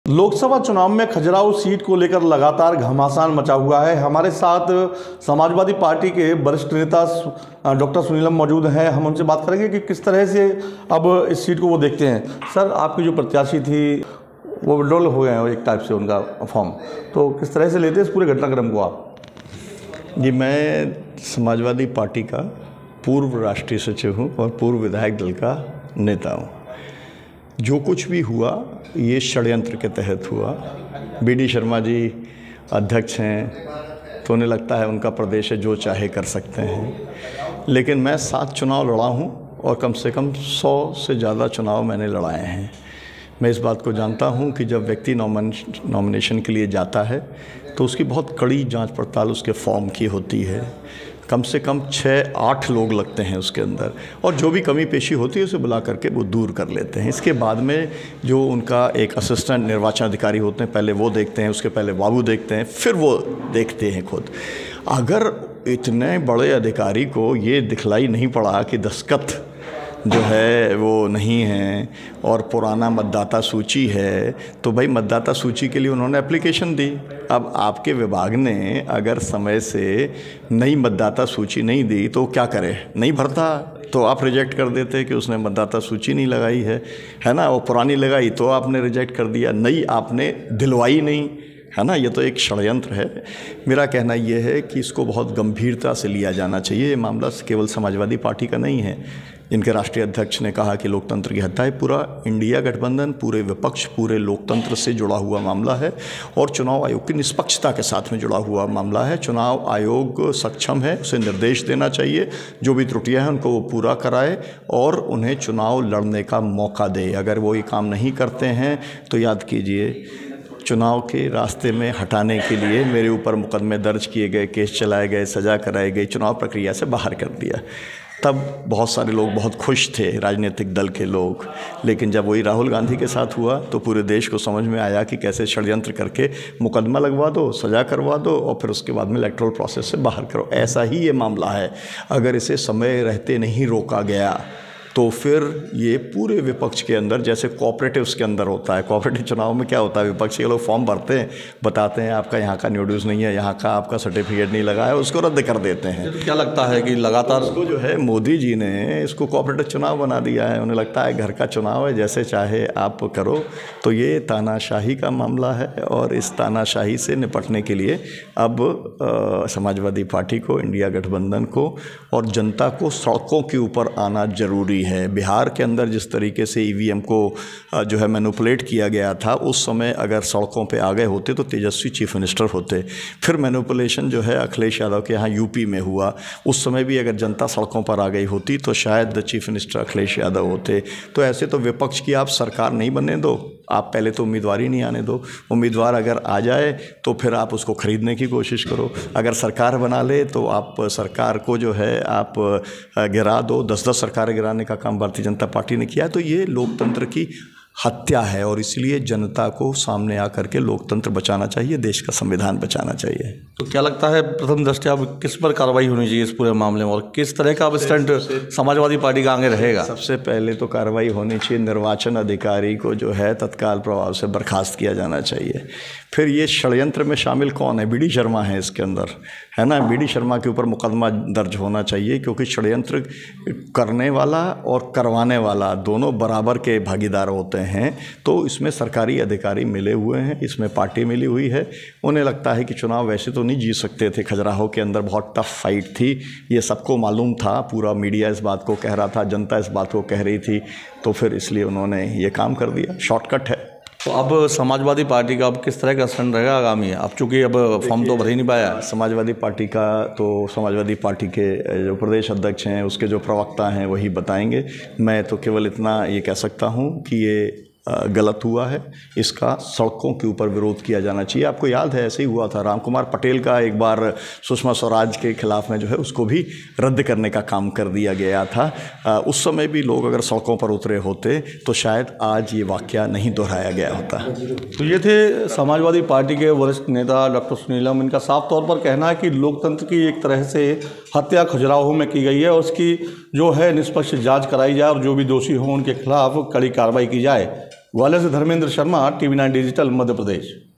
SP leader Sunilam ने VD Sharma पर लगाया Meera Deepak Yadav के खिलाफ षड्यंत्र करने का आरोप
लोकसभा चुनाव 2024 में खजुराहो सीट से समाजवादी पार्टी की प्रत्याशी मीरा यादव का नामांकन निरस्त होने के बाद घमासान मचा हुआ है। इस पूरे मुद्दे पर समाजवादी पार्टी के पूर्व राष्ट्रीय महासचिव, विधायक दल के पूर्व नेता डॉ सुनीलम ने भाजपा सरकार और प्रदेश अध्यक्ष वीडी शर्मा पर षड्यंत्र करने के गंभीर आरोप लगाए हैं
There is an uproar after the nomination of Samajwadi Party candidate Meera Yadav from Khajuraho seat in the Lok Sabha elections 2024 was cancelled. On this entire issue, former national general secretary of Samajwadi Party, former leader of the legislature party, Dr. Sunilam has made serious allegations of conspiracy against the BJP government and state president VD Sharma.